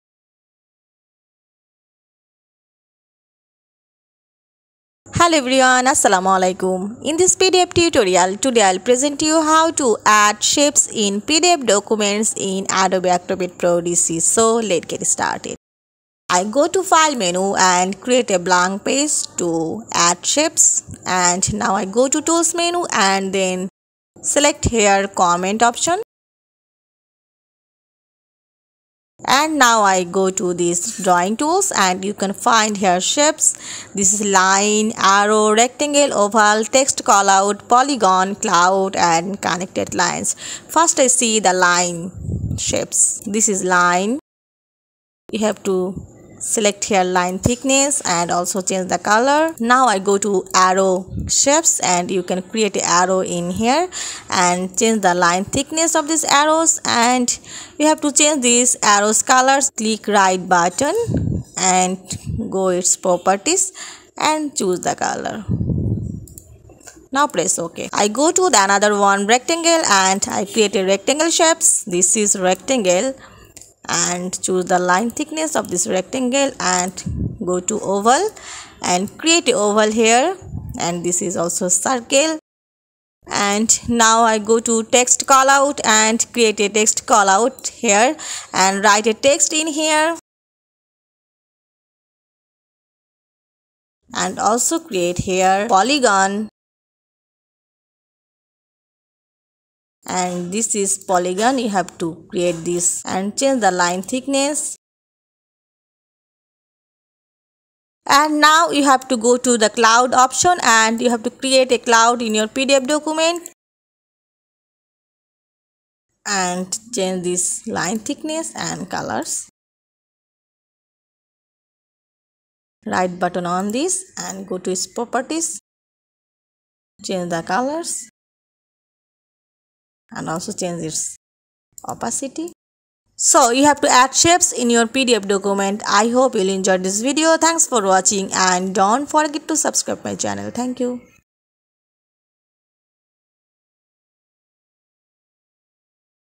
How to add shapes in pdf documents with Adobe Acrobat Pro DC
Hello everyone, Today I will show you, How to add shapes in pdf documents with Adobe Acrobat Pro DC.